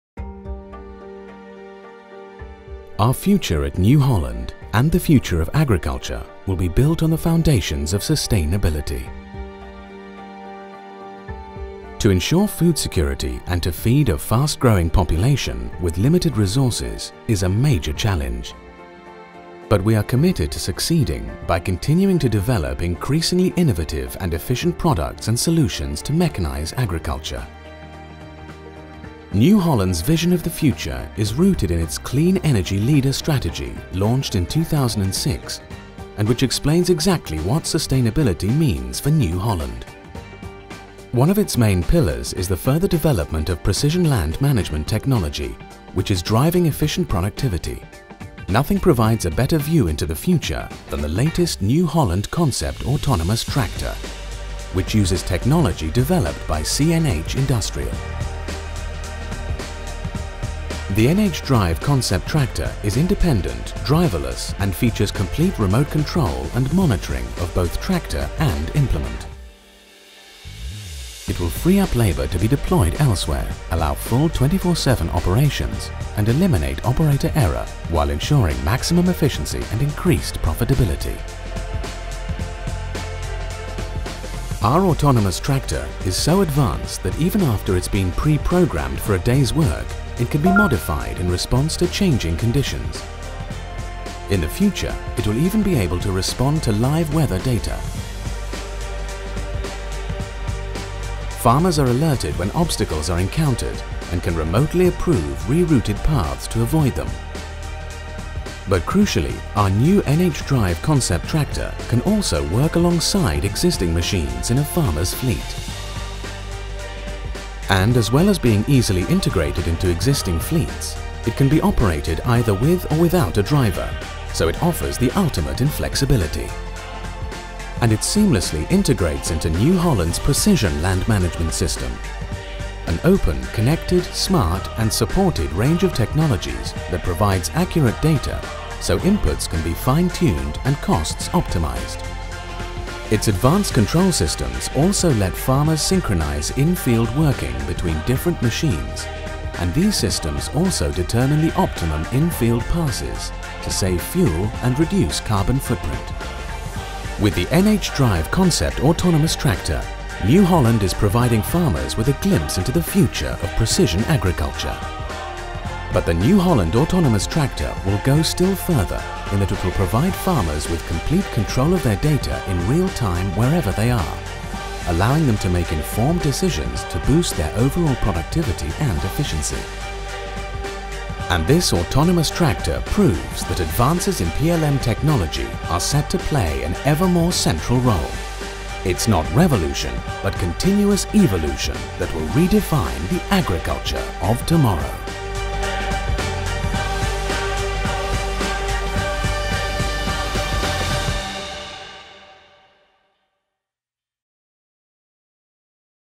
New Holland NHDrive - Concept di trattore autonomo
Il concept di trattore autonomo NHDrive offre al mondo uno sguardo sul futuro dell’agricoltura. Permette di ottenere la massima flessibilità essendo capace di lavorare 24 ore su 24, 7 giorni alla settimana, con o senza un operatore grazie alla cabina del T8. È perfetto per la supervisione delle operazioni automatizzate in campo o il controllo e monitoraggio da remoto, e può viaggiare autonomamente su percorsi privati pre-mappati. Il concept di trattore autonomo NHDrive mette a frutto tutta l’avanzata tecnologia New Holland PLM (Precision Land Management) per il precision farming, e può essere perfettamente integrato nel parco macchine esistente.